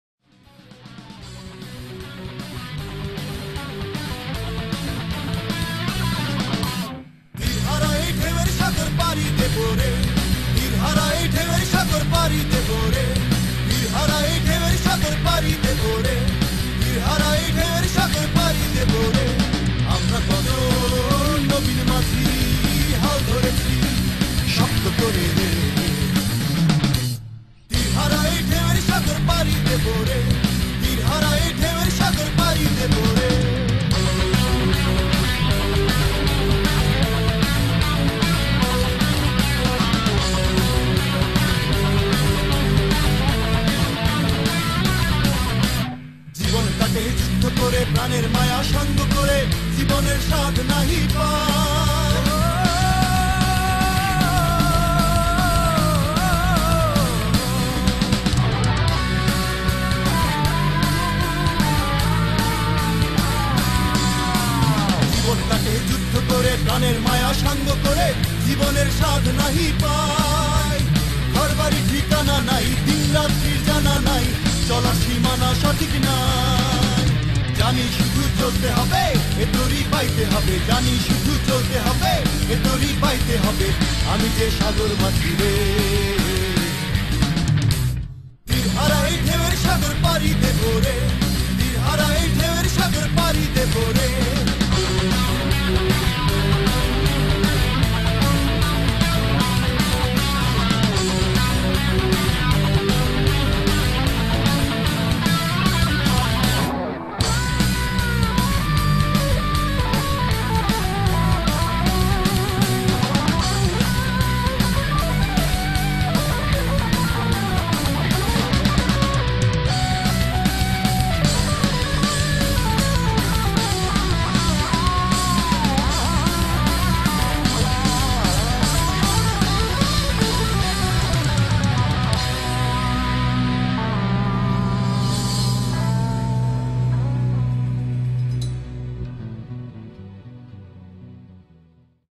McGill BSA: We Stand With Shahbagh
McGill Bangladeshi Students Association: We Stand With Shahbagh
Date: 9th February, 2013
Location: McGill University, Montreal, Canada